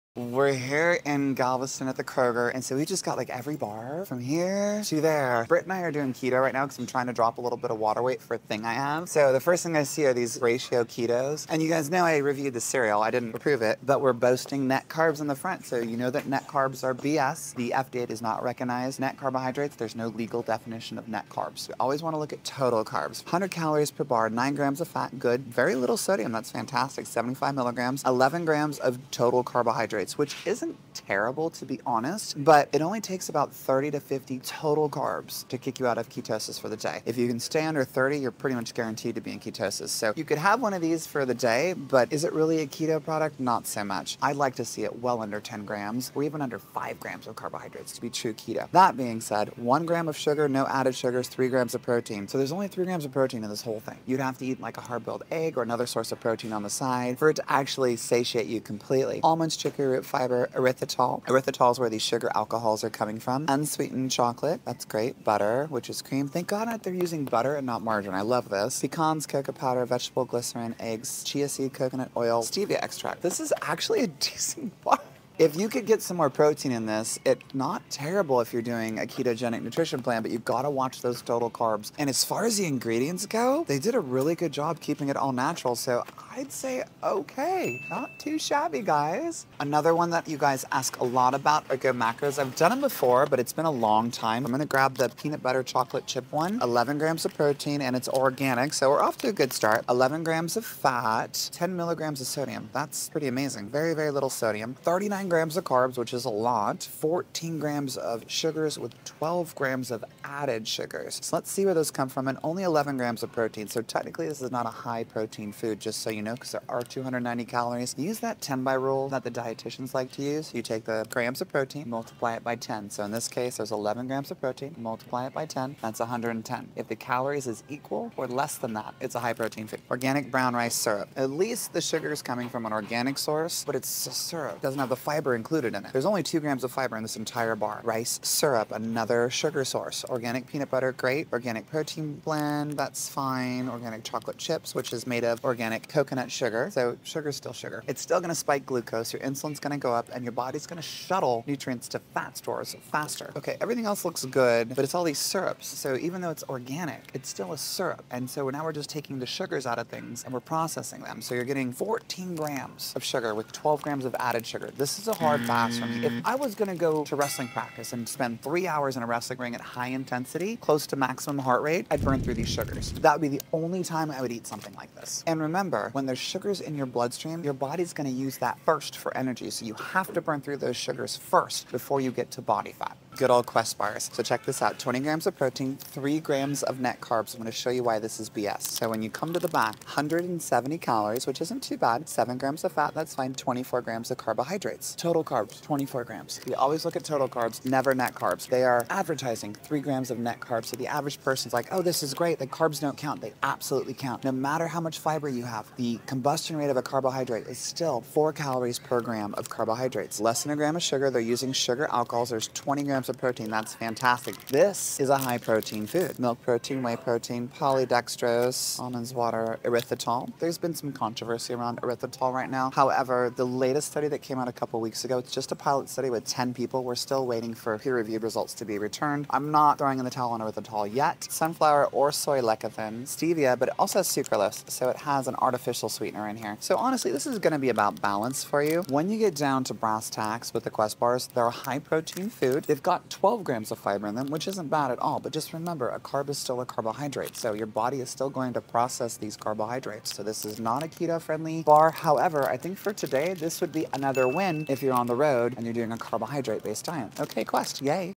2 wins and a pass for these protein bars
2 wins and a pass for these protein bars. - Added sugars, to any foods, if not used for energy immediatley will get shuttled faster to body fat. - Sugars are quick energy and ingesting them without immediately exercising will provide a short burst of energy BUT the leftover glucose in your bloodstream that isn't needed, insulin will suttle that to your long term energy source - AKA fat. - Also, it's important to know that your body tends to use glucose (blood sugar) first as it's fuel for exercise. So if you're loading up on sugars prior to exercising and hoping to drop pounds, know that your body must first use the sugars you ingested BEFORE it begins to utilize your body fat stores. - Remember, I never judge your choices, and I'm only here to help provide information so that you can see the best progress for YOUR personal goals. - Details in my profile for my one on one fitness and nutrition coaching… – Or go to coachgabbi.com. – Happy shopping!